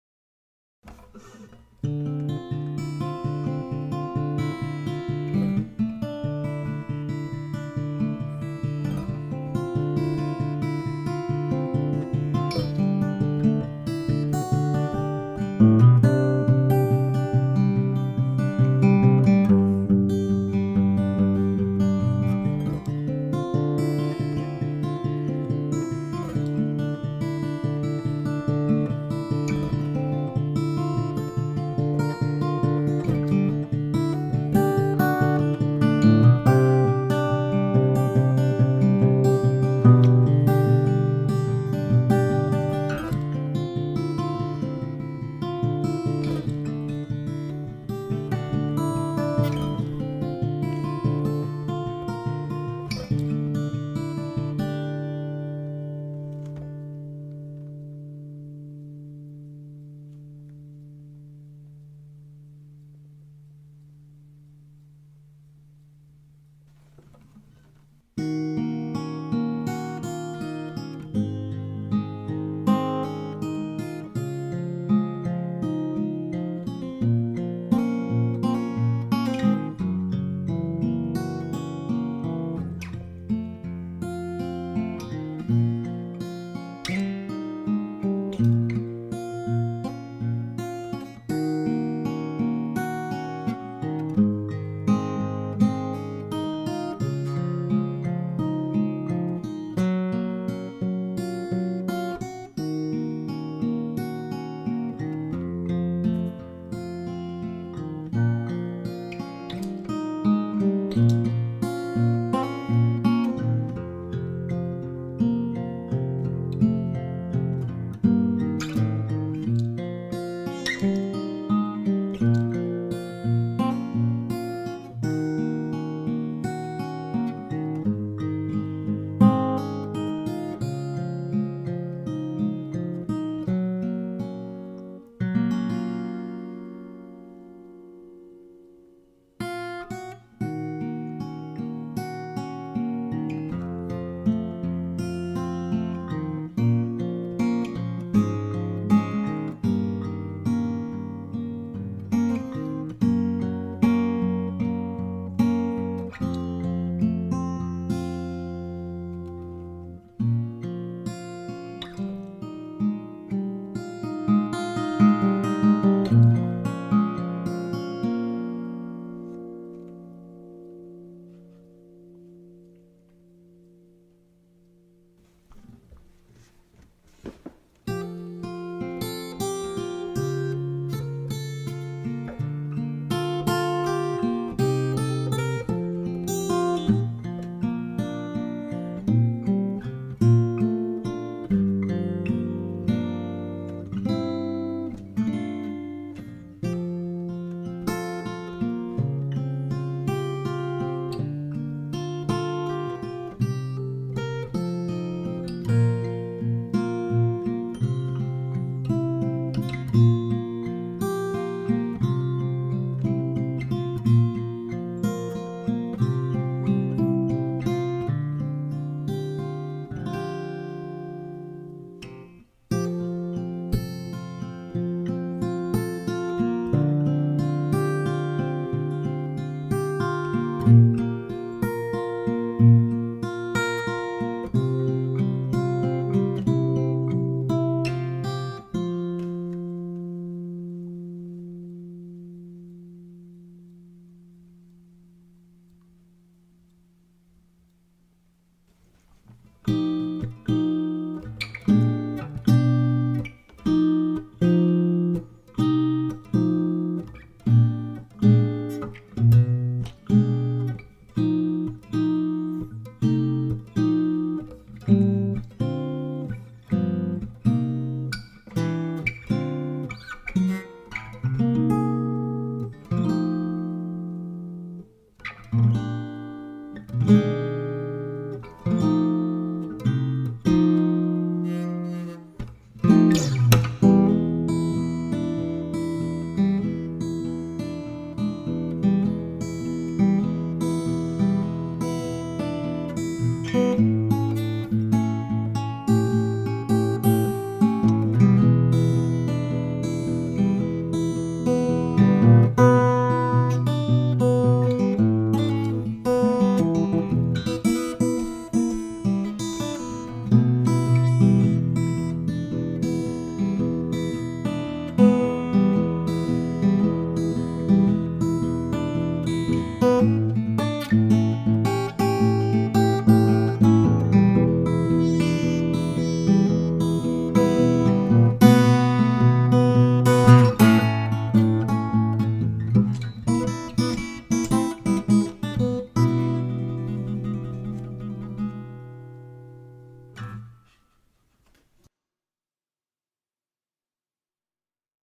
Martin 00-15. Great Audio!
Various tunes played on my Martin 00-15.

Recorded with a single ADK A51e. A little reverb added afterwards.